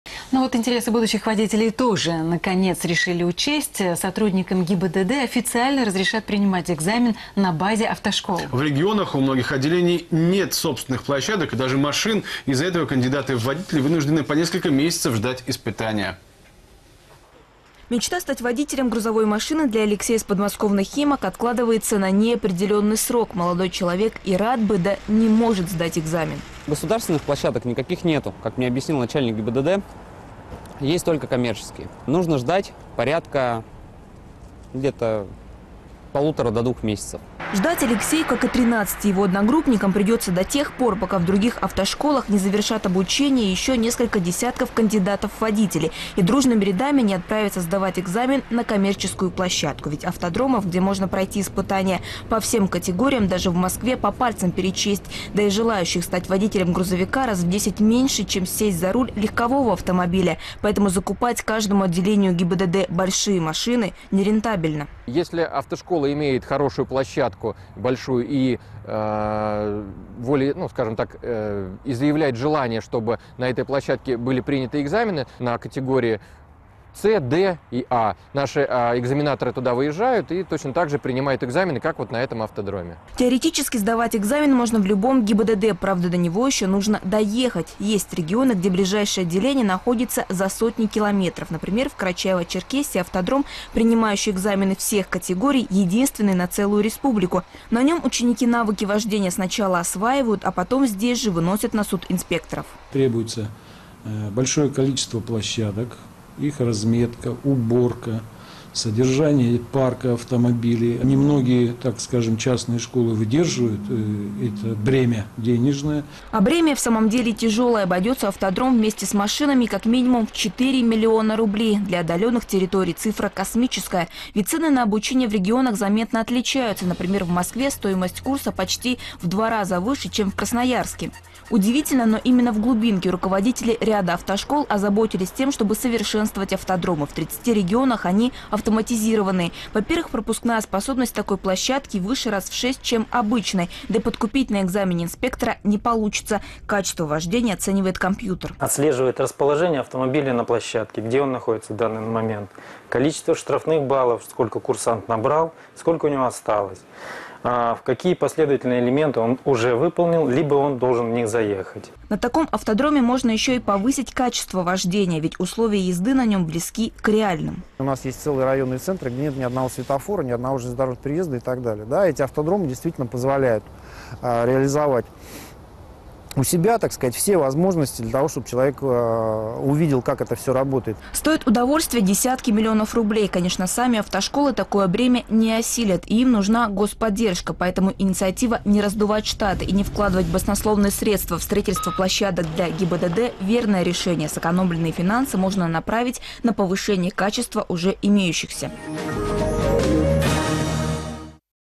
ГИБДД и автошколы: актуальные вопросы приема экзаменов
Сотрудникам ГИБДД официально разрешат принимать экзамены на базе автошкол.

В регионах у многих отделений ГИБДД нет собственных площадок и даже машин, из-за этого кандидаты в водители вынуждены по несколько месяцев ждать испытания...